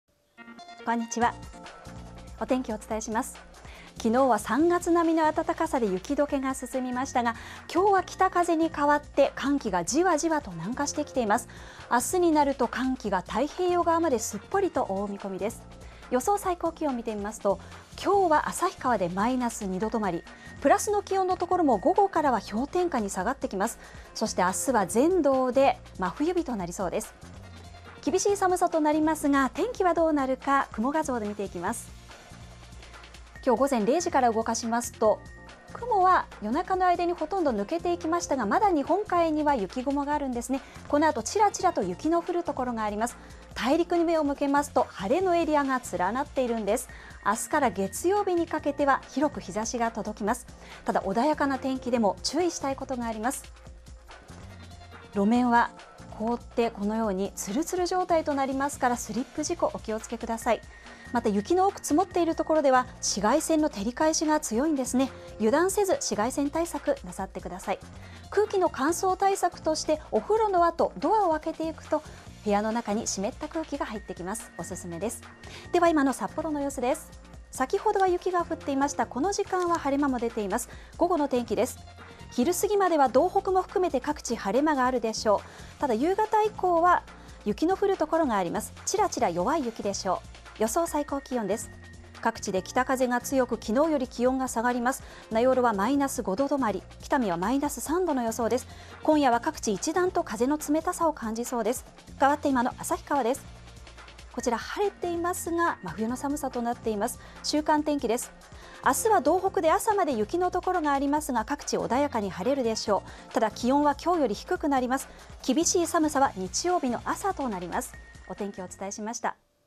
２２日（金）北海道の天気 ポイントは「北風とともに気温ダウン！雪のちらつく所も」【HTB北海道ニュース】
24時間ノンストップのニュースLIVE「北海道ニュース24」